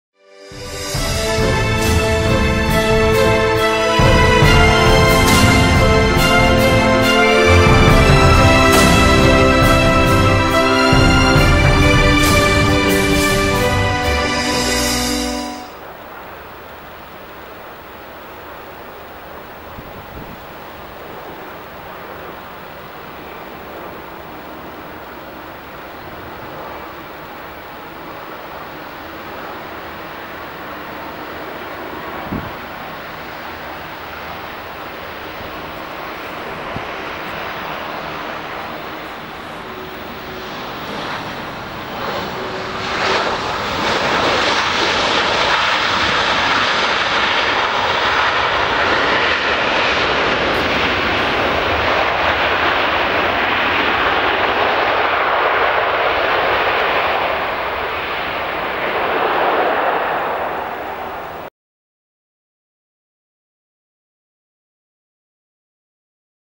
Boeing 737-800 long landing in Luxembourg
In this video you can see the landing of a Boeing 737-800 of Luxair [LX-LGU] in Luxembourg on runway 24. The plane is 1.2 years old and is the MSN 41047.